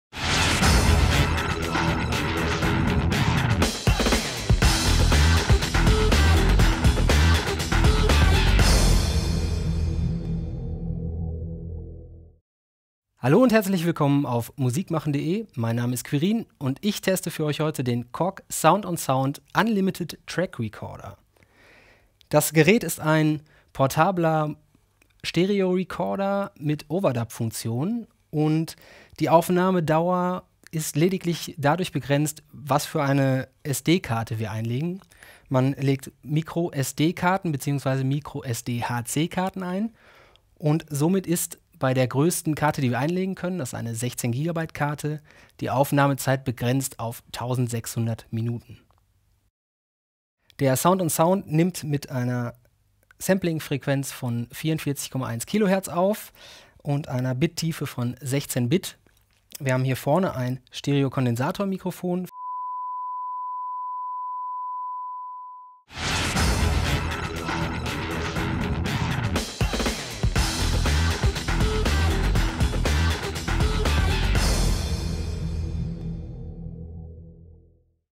Korg Sound on Sound im Test auf MusikMachen.de
Unlimited Track Recorder -- so der Zweitname des neuen handlichen Korg Sound on Sound, der unter seinem kleinen schwarzen Gehäuse doch tatsächlich ein tragbares Musikstudio komplett mit Drummachine, Effekten und Overdubbing beherbergt. Das unbegrenzte Aufnahmeerlebnis wird dabei durch das Einschieben einer microSD- bzw. microSDHC-Karte von bis zu 16 GB ermöglicht, und das zu guter Klangqualität. Das sind Parameter, die förmlich nach einem Test auf MusikMachen.de schreien.